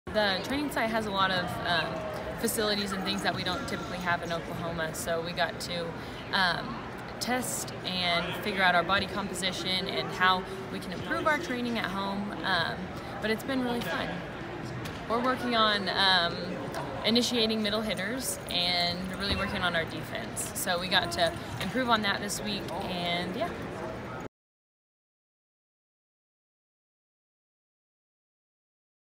Kaleo Maclay on the Olympic Training Center | USA Volleyball
Kaleo Maclay and the U.S. Women's National Sitting Volleyball Team spent a week of training camp at the Olympic Training Center in Colorado Springs, Colorado. Kaleo shares what she loves best about training there!